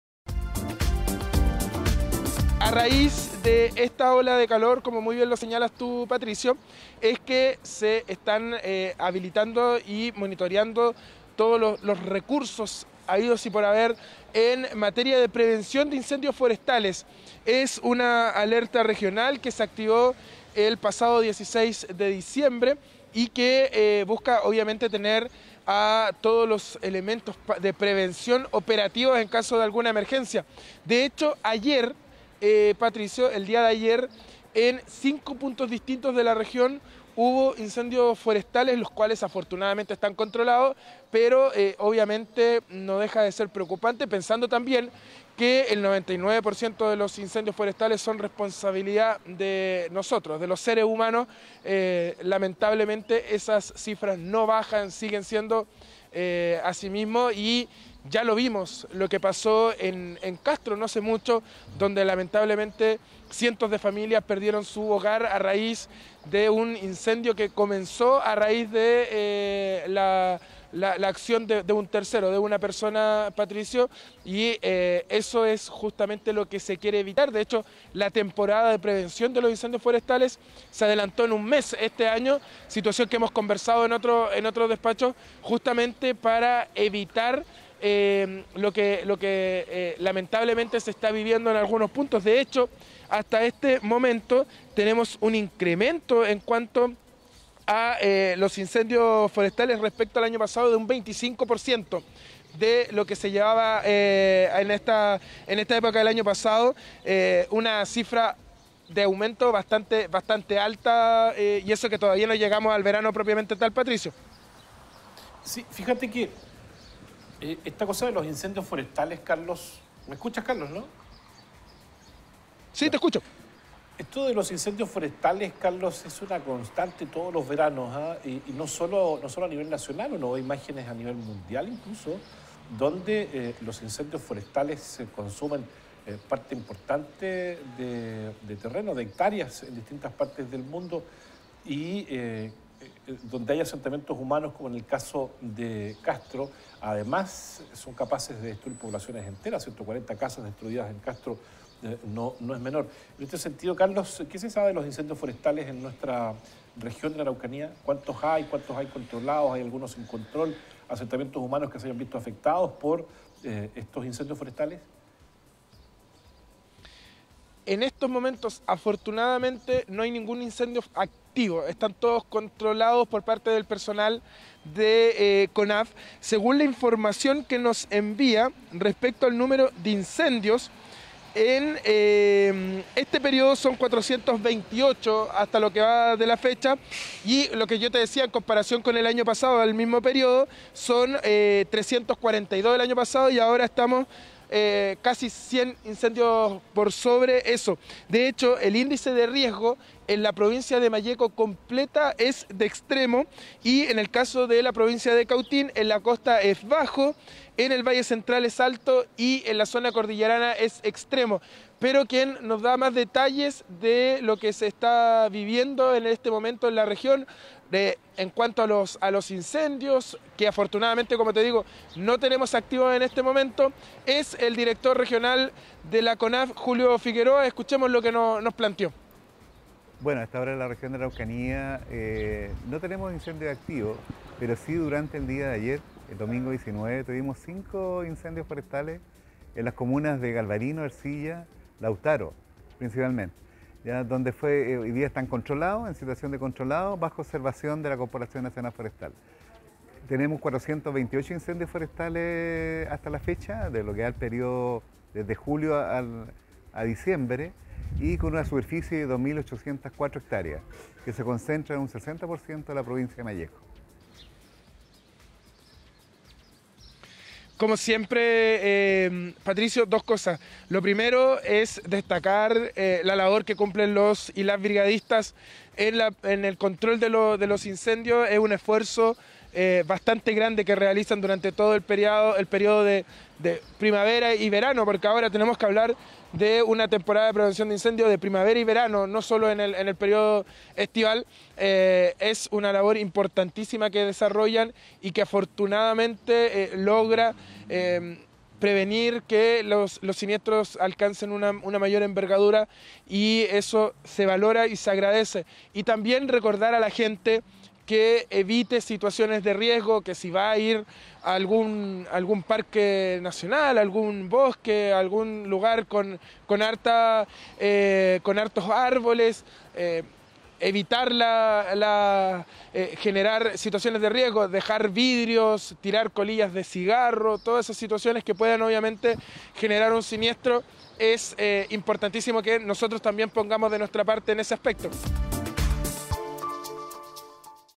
Ola de calor en La Araucanía: Incendios forestales han aumentado en un 25% | ARAUCANÍA 360°
Frente a la ola de calor y al aumento de incendios forestales en un 25% en comparación al año 2020 en La Araucanía, la Corporación Nacional Forestal está llevando a cabo un monitoreo constante en materia de prevención. Así lo señaló el director regional de CONAF, Julio Figueroa, quien indicó que entre junio y diciembre del presente año se han registrado un total de 428 incendios forestales con una superficie afectada de 2.804 hectáreas.